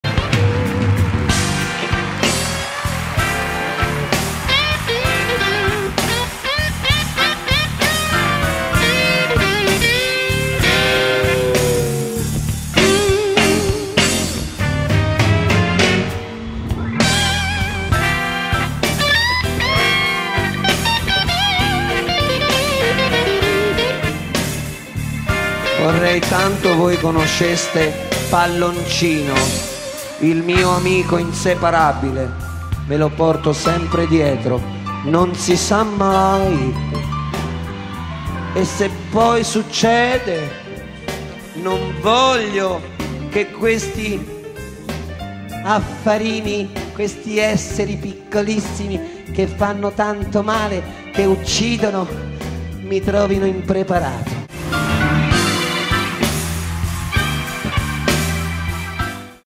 RENATO ZERO E IL PALLONCINO (Live)
RENATO ZERO E IL PALLONCINO estratto del concerto L'Imperfetto Assago Milano , breve  dialogo ironico e divertente sul sesso protetto di Renato  (Live) video archive by De Simone Giacomo